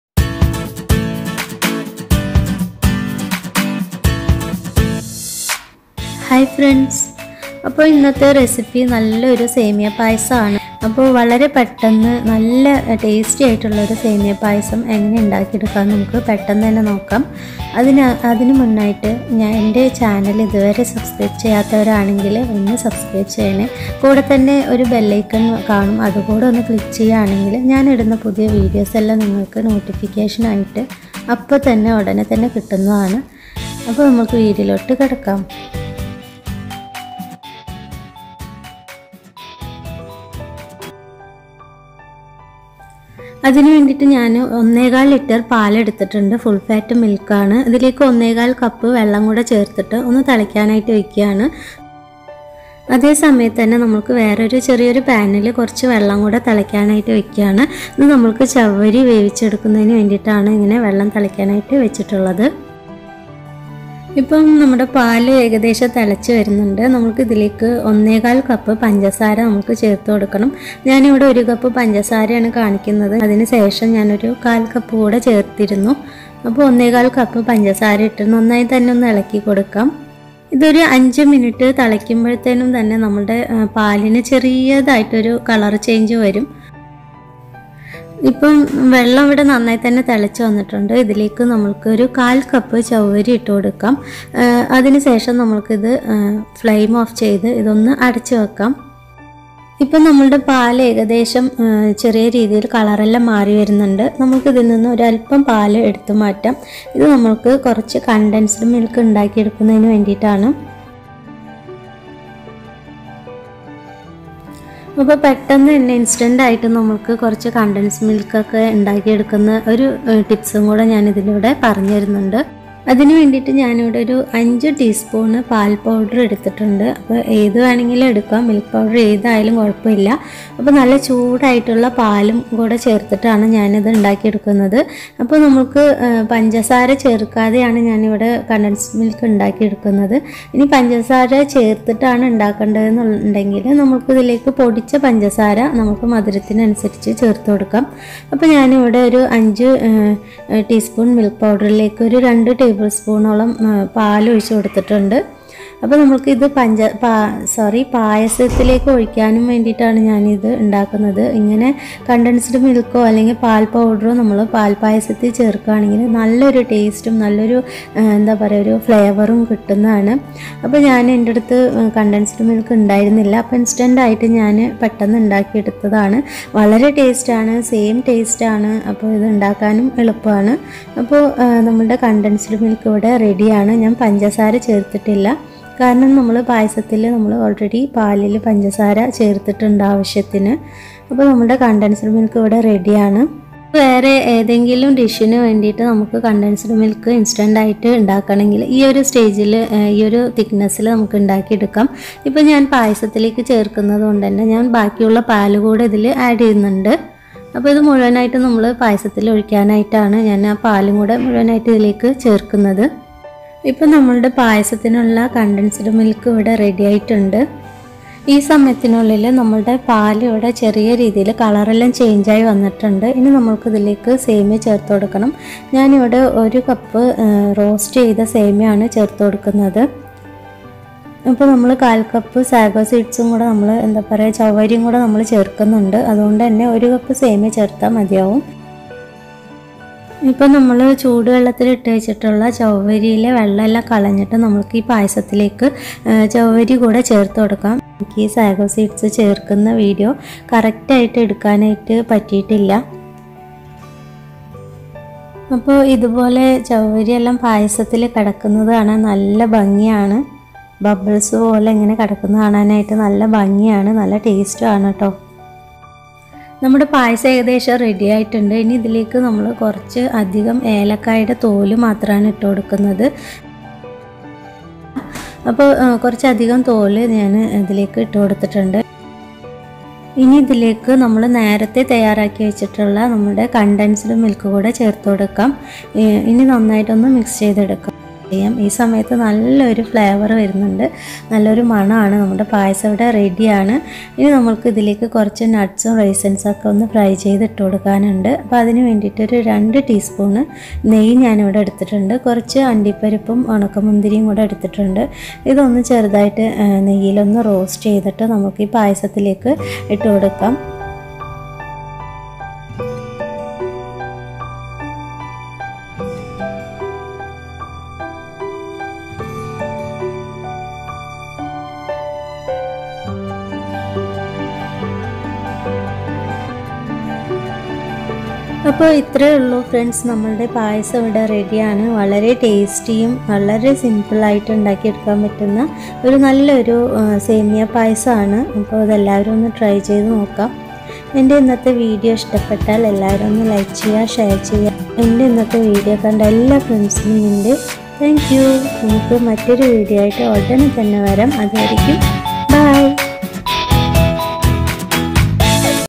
How to Make Semiya Payasam||സേമിയ പായസം ഇതുപോലെ ഒന്ന് ഉണ്ടാക്കി നോക്കു||Easy & Tasty Semiya Payasam
Ingredients:

Vermicelli (semiya) 1 cup
Sago seeds(ചവ്വരി) 1/4 cup
Milk  1.1/4 L
Milk powder 5 tsp (full)
Cardamom
Raisins
Cashew nuts

Hope you guys try this out at home. Have a good day.